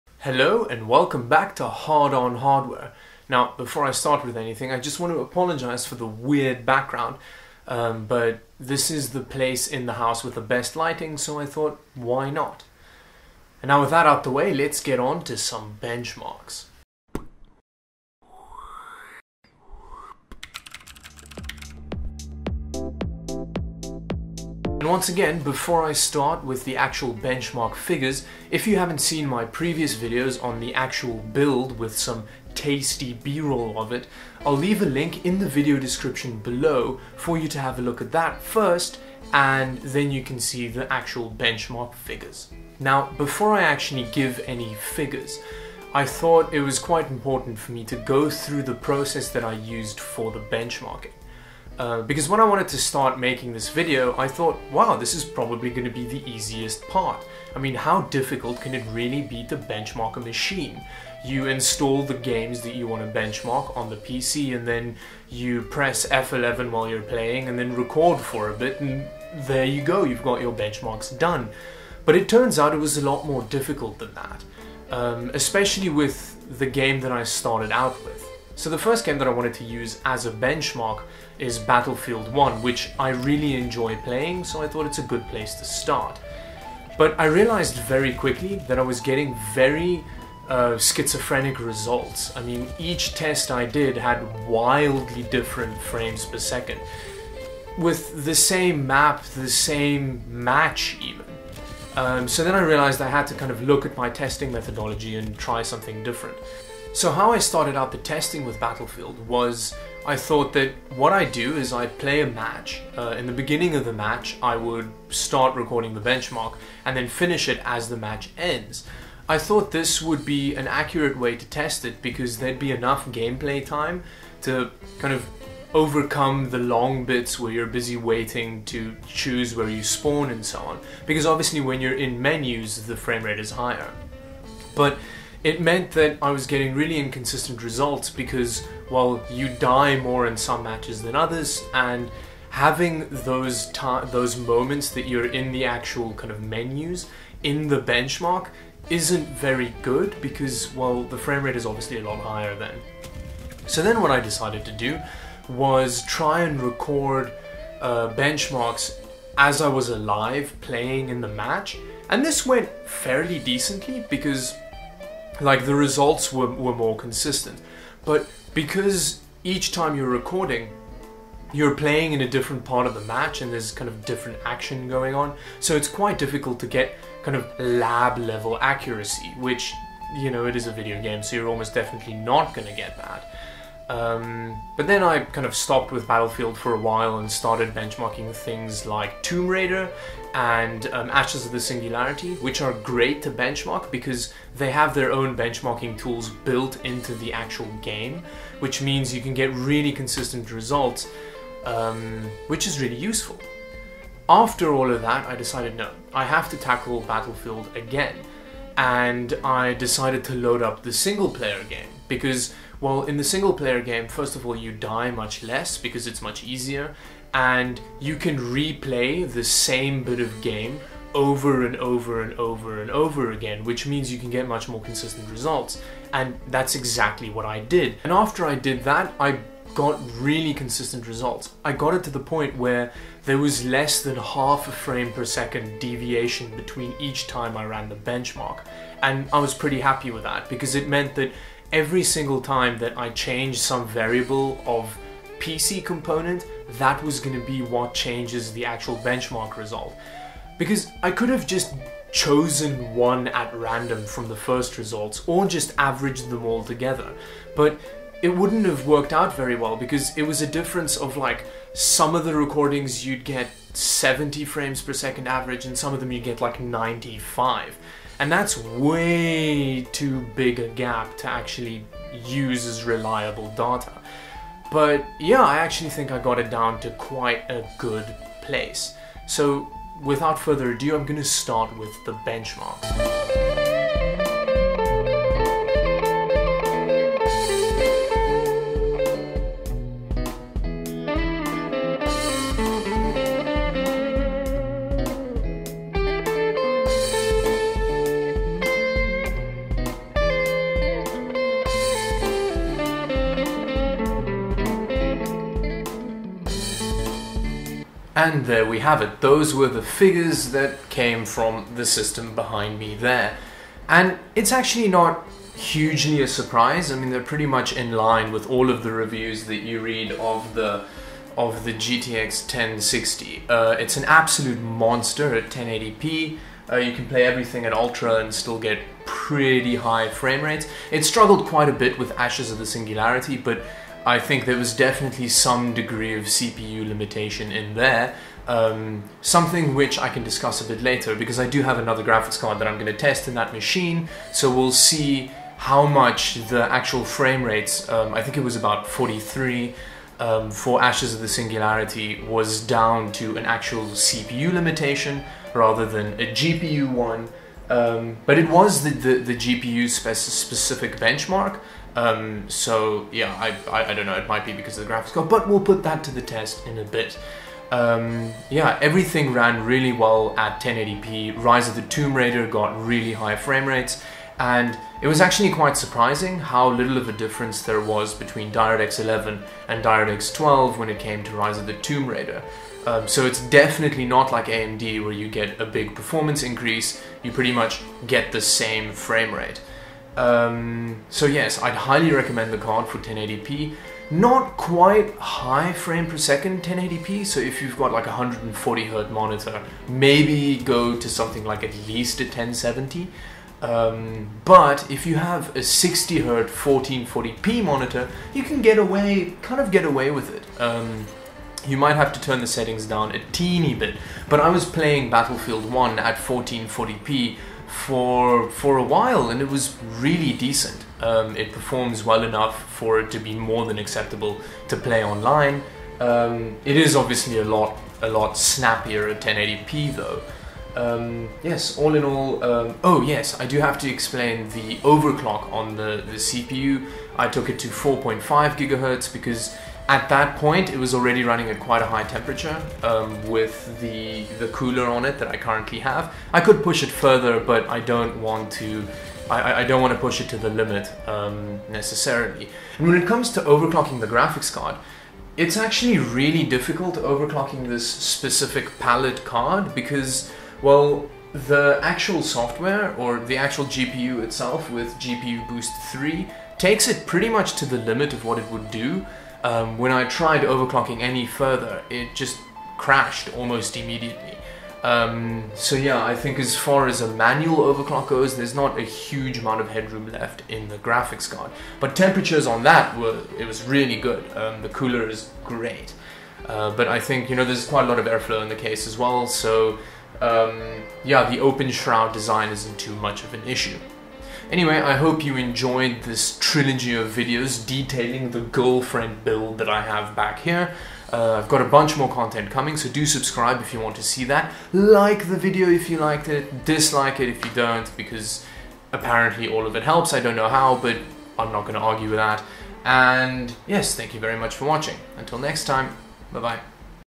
Girlfriend build 3: The benchmarkening (The trilogy draws to a close)
All good things come in threes: Great movie trilogies, amounts of musketeers, and groups of sexual partners. That is why a set of three videos is the best way to thoroughly take advantage of the girlfriend build.